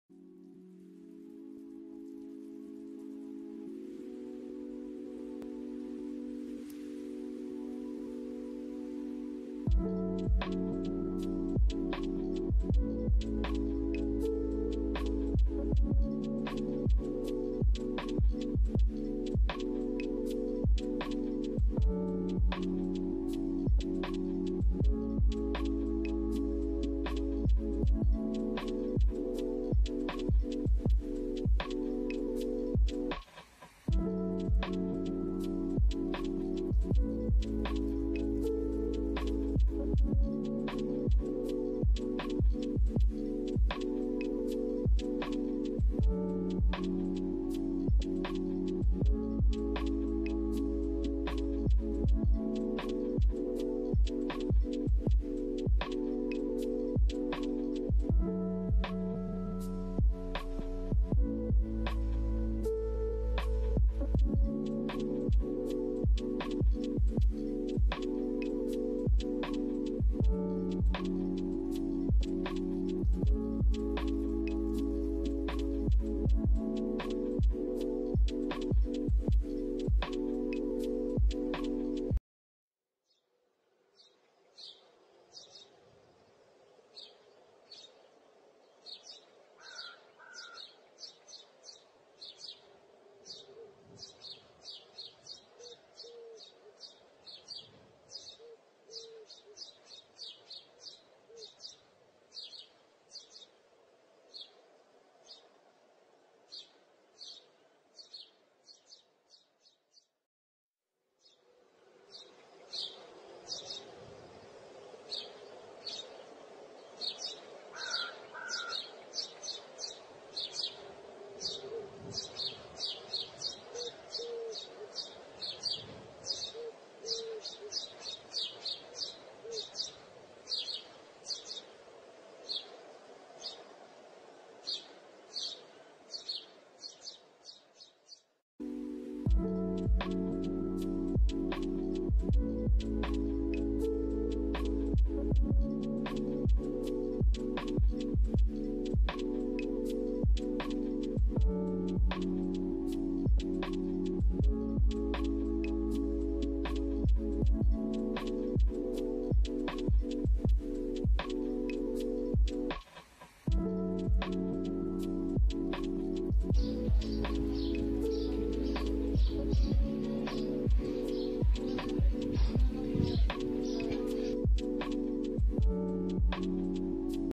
Story Of A Bird Who Wants To Fly/ Injured rescue Bird/ Babies with beauty
This Story is one of my little rescued friend, who wants to fly, he fights for days but give up at the end and died.
Sorry to this little soul, i'll miss You forever my friend ❤